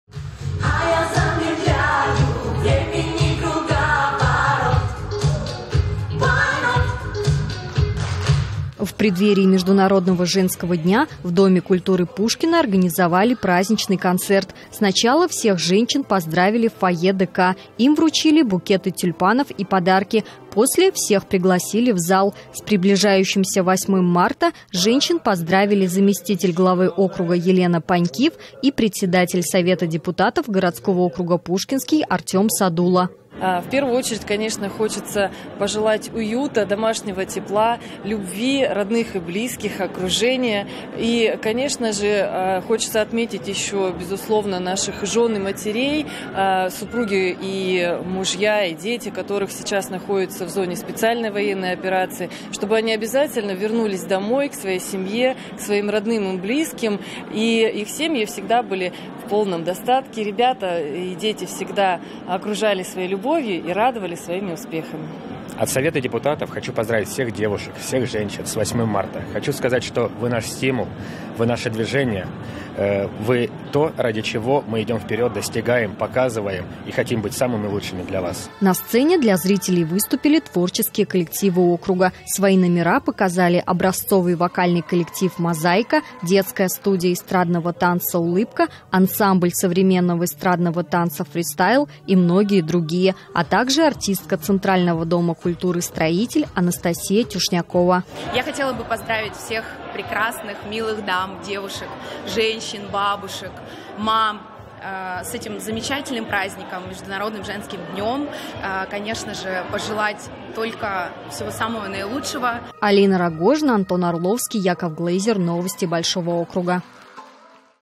Всё для милых дам. В Доме культуры «Пушкино» женщин поздравили с 8 марта
В Доме культуры «Пушкино» женщин поздравили с наступающим Международным женским днём. Каждой вручили букеты тюльпанов и подарки.